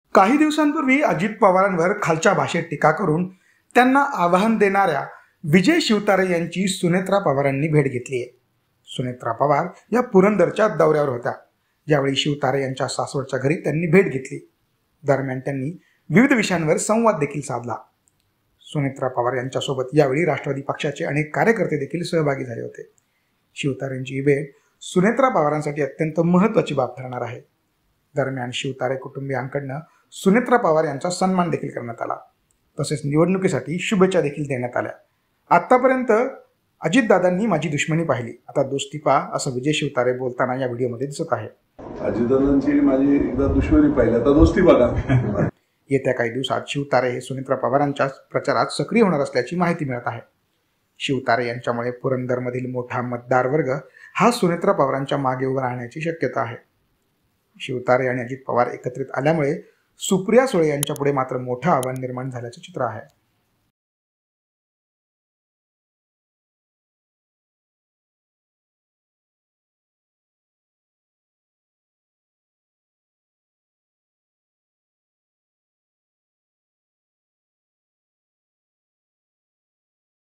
Sunetra Pawar : सुनेत्रा पवार शिवतारेंच्या घरी... पुढं काय घडलं? | LetsUpp Marathi
बारामती लोकसभेच्या प्रचाराच्या अनुषंगाने सुनेत्रा पवार यांनी माजी मंत्री विजय शिवतारे यांची भेट घेतली.